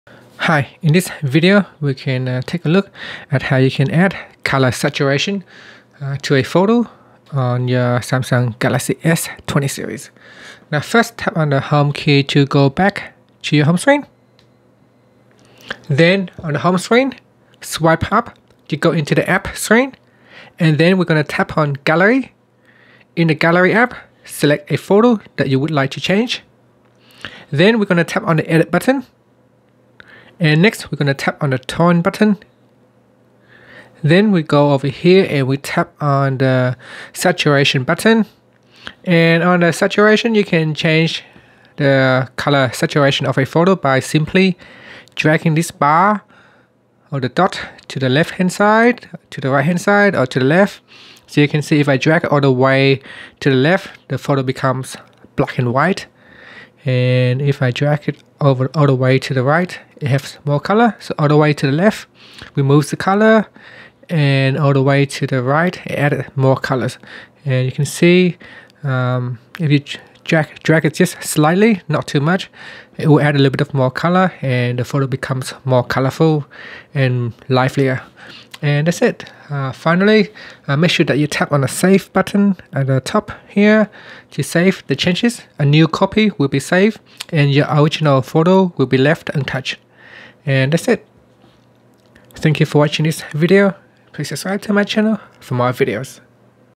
Galaxy S20/S20+: How to Change Color Saturation of a Picture
Learn how you can change color saturation of a picture on Galaxy S20 / S20 Plus / S20 Ultra.

Android 10.